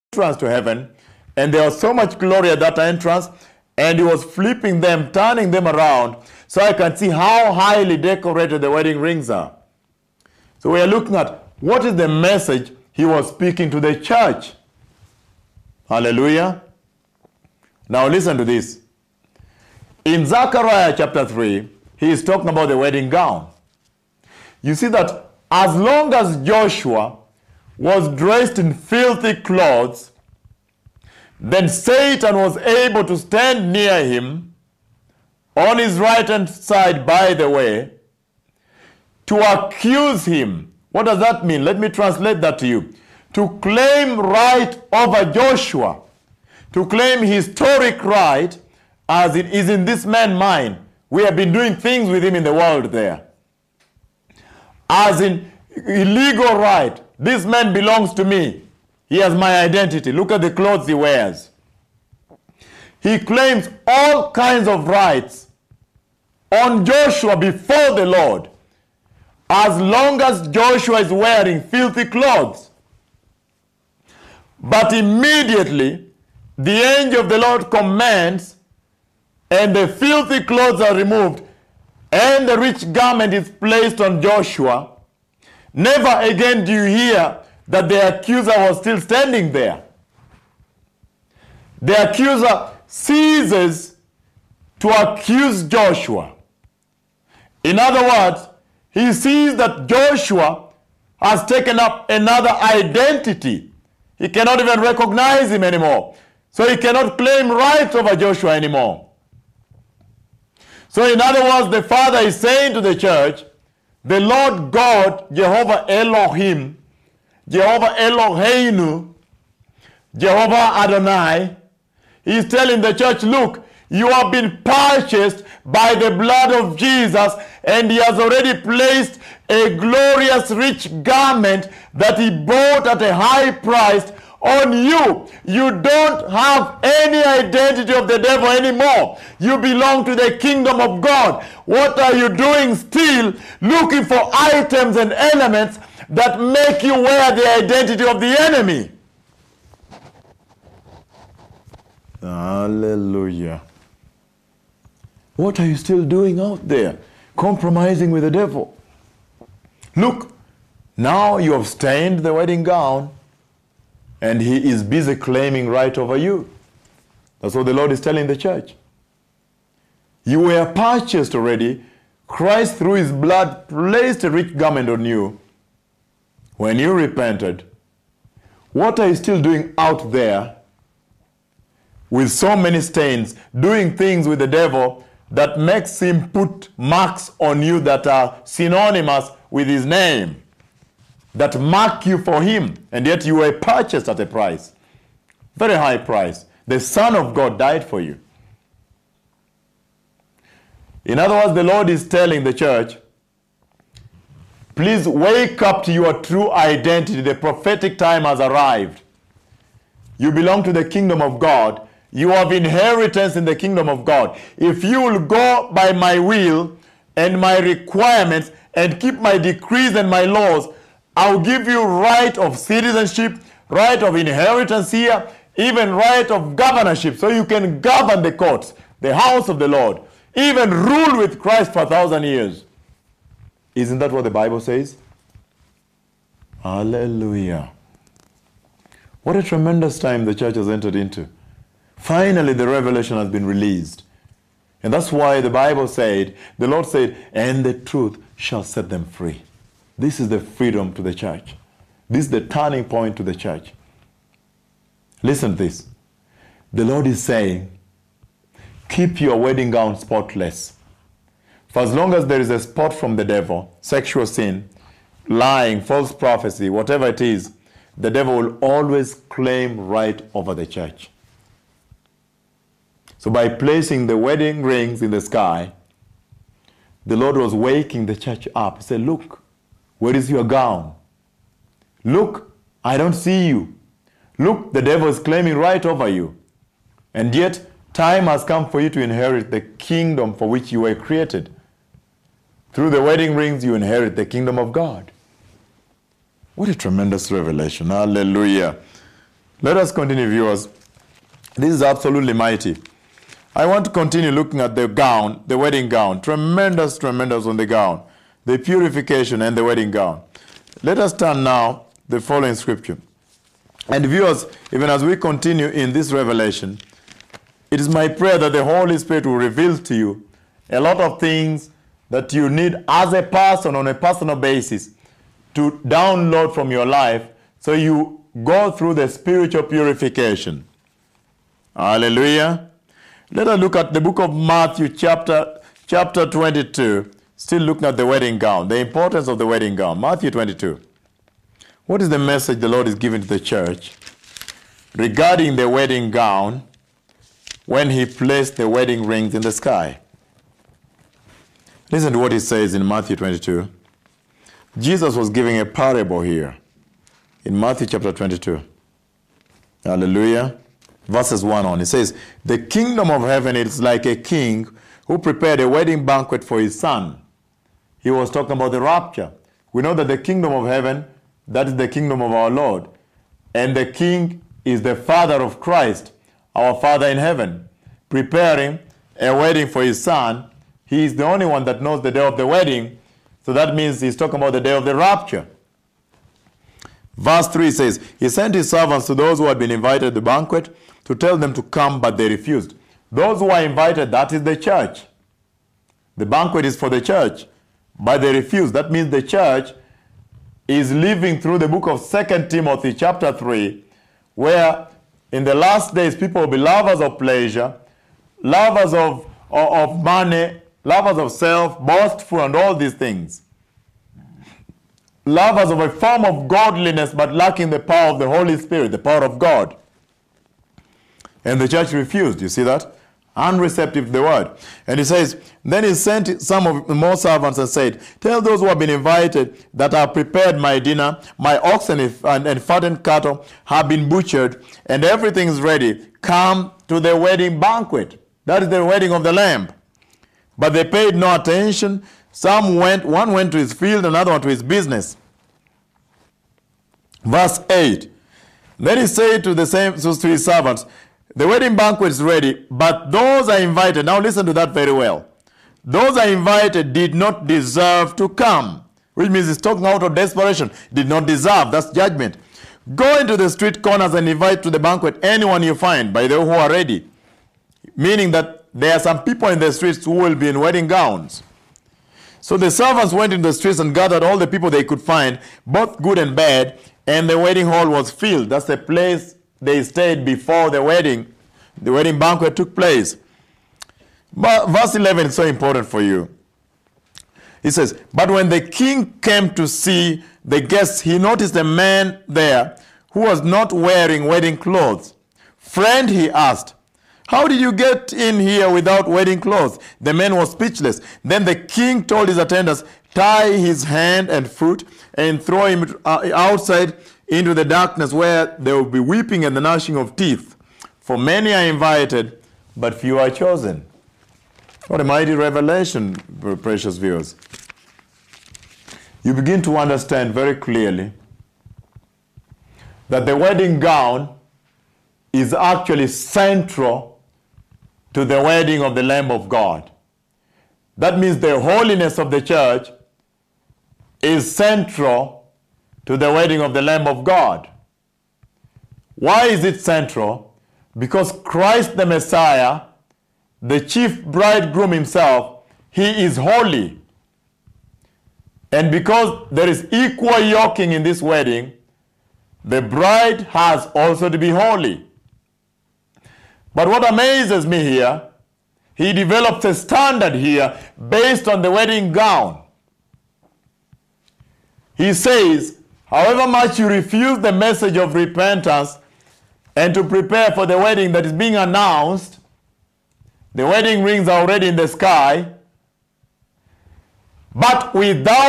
The Glorious Wedding Gown (part 5 final) Prophet Dr. Owuor!!
Powerful, Life changing Message of the LORD repeated for nearly a decade that has not changed! Let us prepare our Garment of the LORD in righteousness, and holiness for the Wedding Supper of the Lamb is nigh!!

Shalom!